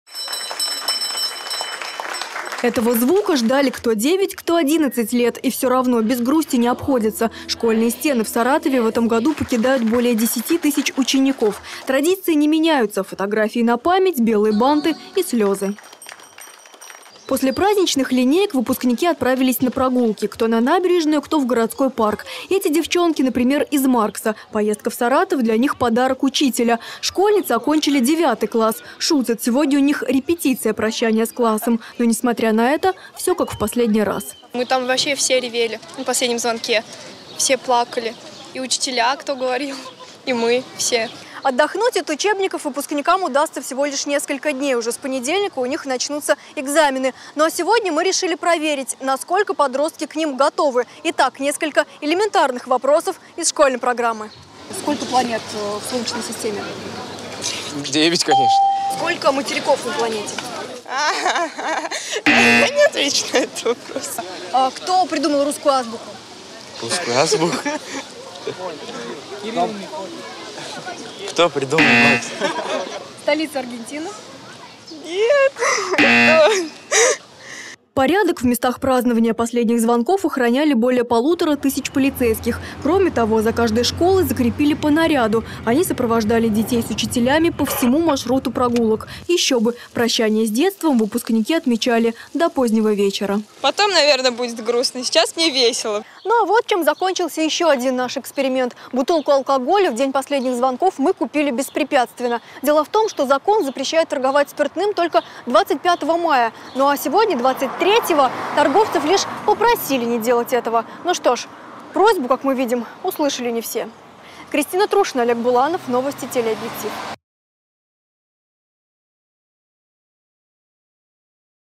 Последний звонок [ВИДЕО]
Они уже не дети, не еще не взрослые. В минувшие выходные для саратовских выпускников прозвенели последние звонки. В этом году школьные стены покидают более десяти тысяч школьников. По традиции с утра молодежь поздравляли в родных классах. Ну, а после официальных церемоний подростки отправились гулять. Прощание с уроками отмечали до позднего вечера...
Кристина Трушина школу УЖЕ давно окончила, поэтому решила освежить воспоминания.